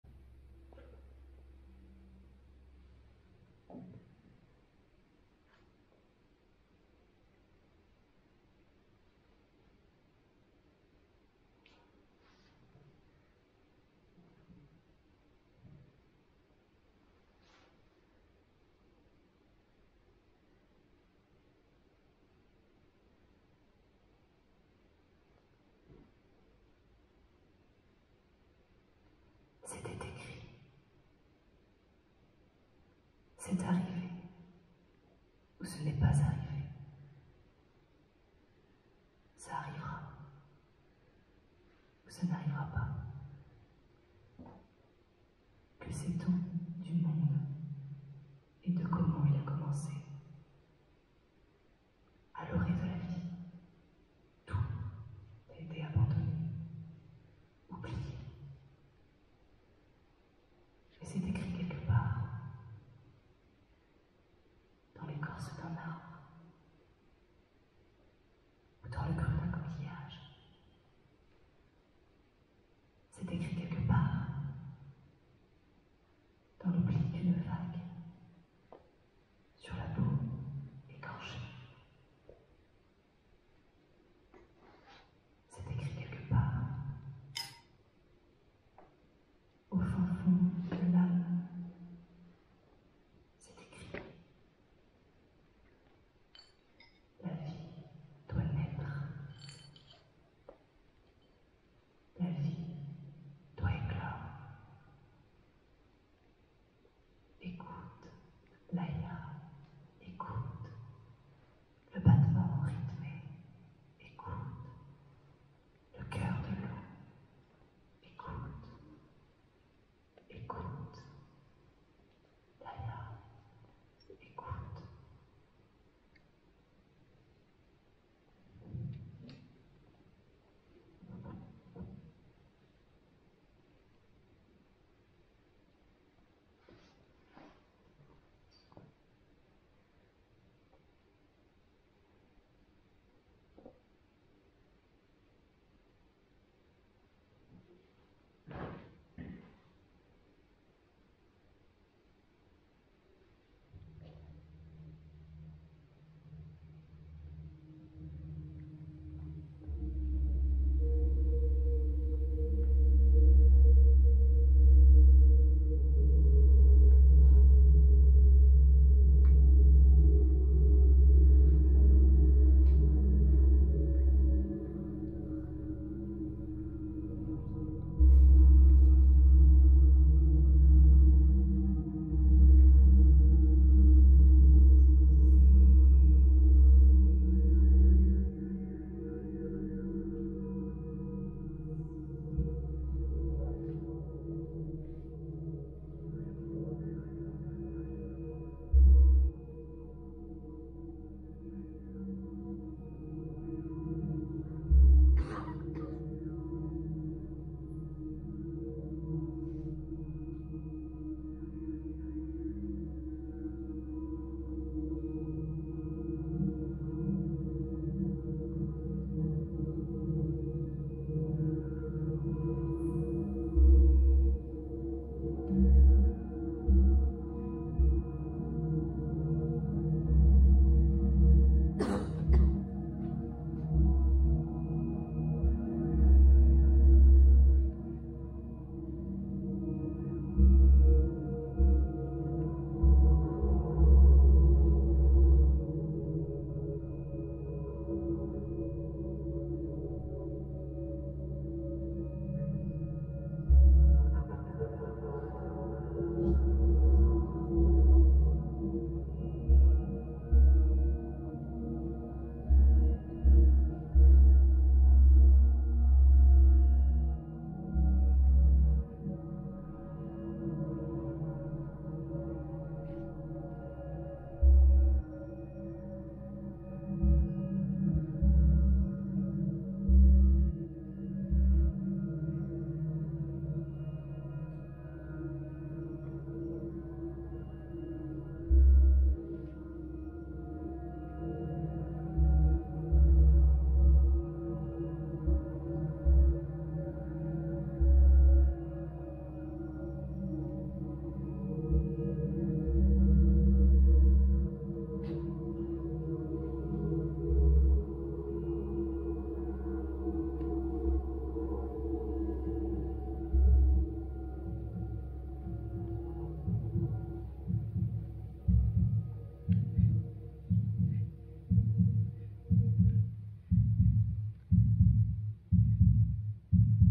Extrait de répétition de "Nais-Sens" résidence à la boîte juin 2021
Laïa découvre tour à tour que son monde peut être dangereux car le prédateur veille sous la forme d'un serpent, il peut être savoureux aussi grâce à la nature luxuriante et aux fleurs géantes qui la rassasie. Il peut être curieux, fascinant également quand le bruit d'un coeur qui bat à l'intérieur d'un oeuf, qu'elle trouve, la pousse à grandir, à comprendre que le but même de la vie c'est ce qui pourrait être dans cet oeuf...Mais qu'est-ce ?
Spectacle de marionnettes pour tout public à partir de 8 ans
Ecrit et lis en scène par Laurence Stajnowicz
Remanier par Laurence, Marion et Léa. Décor : Crako
Régie : Mikay et Oriane
Bruitage : Léa